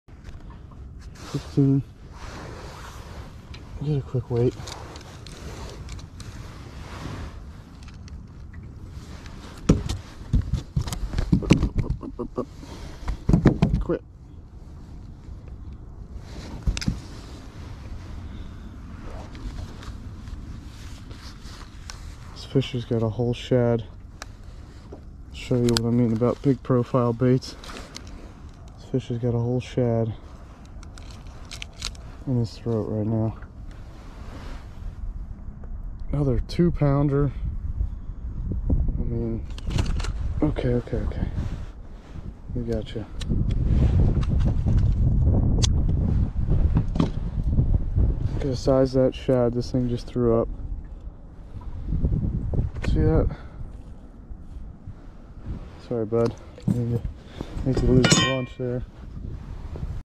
Late Fall is not the time to downsize your crappie baits.
Use 2.5 to 3.5 inch swim bait/paddle tail type plastic jig. My favorites are the Hazedong shad by Megabass and the Slab Magnet by Crappie Magnet/Lelands Lures